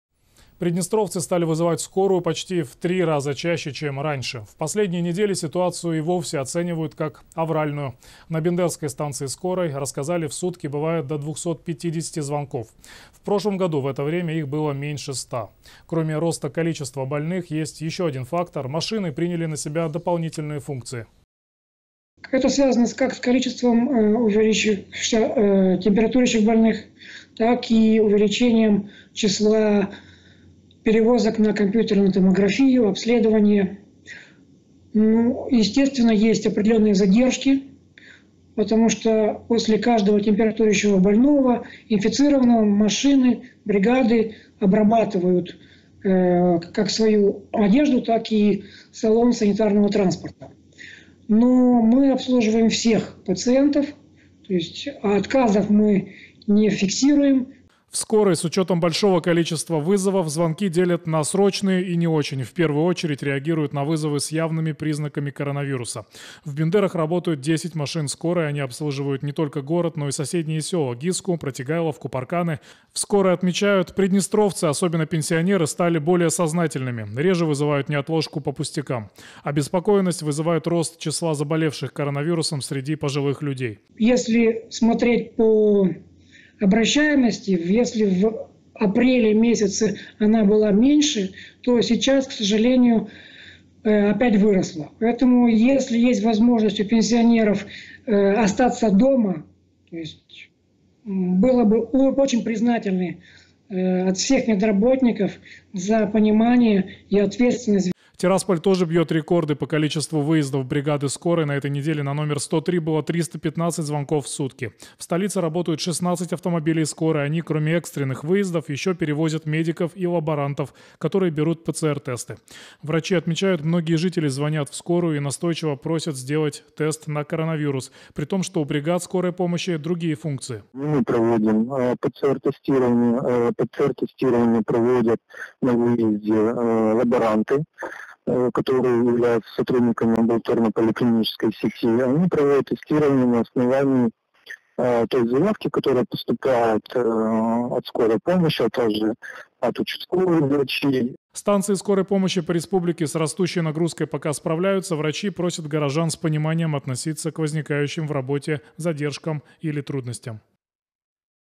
Антирекорды скорой помощи
Приднестровцы стали вызывать «скорую» почти в три раза чаще, чем раньше. В последние недели ситуацию и вовсе оценивают как авральную. На Бендерской станции «Скорой помощи» рассказали, в сутки бывает до 250 звонков. В прошлом году в это время их было меньше ста. Кроме роста количества больных есть ещё один фактор. Машины приняли на себя дополнительные функции.